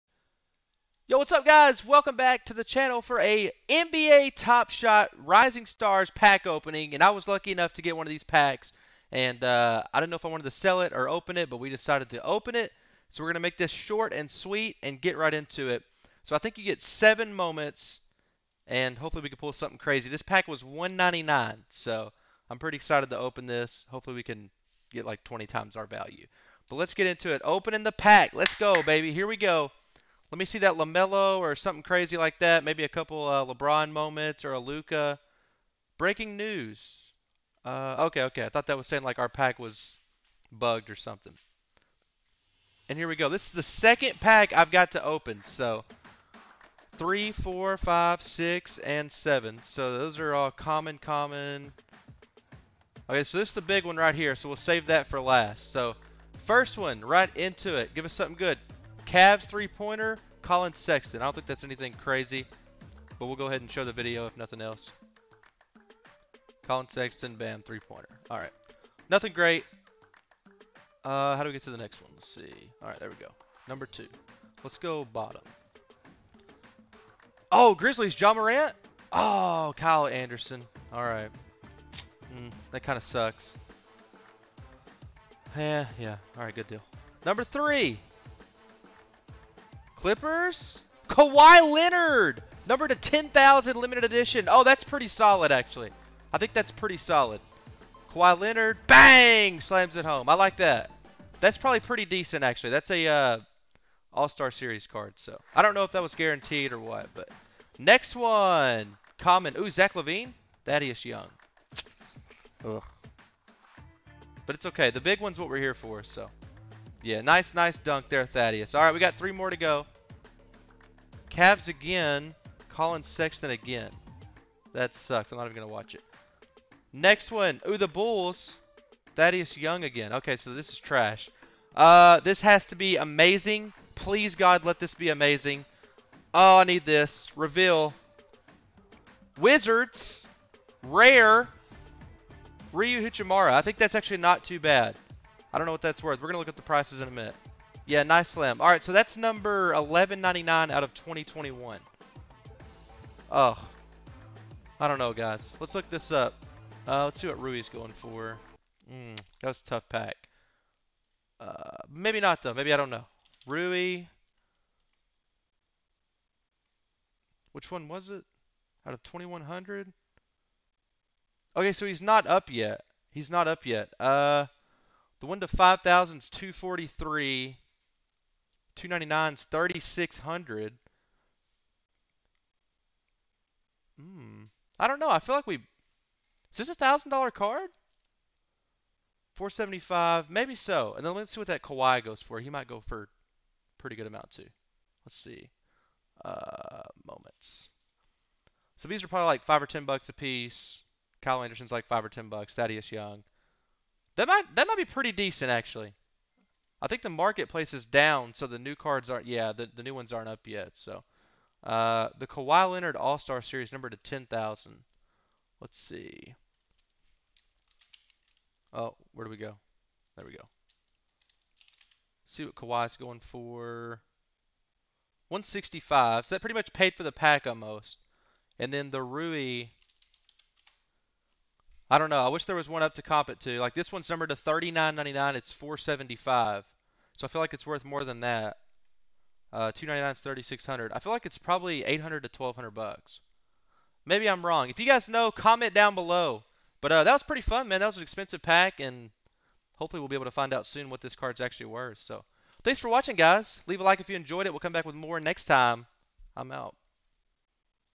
NBA TOPSHOT RARE RISING STARS PACK OPENING!! $199 PACK!!
we are opening one of the hot all new nba top shot rare rising star packs in todays video!